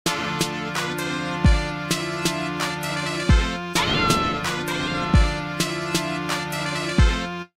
Audio test 2
rap beat sounding thing I made with some nintendo sounds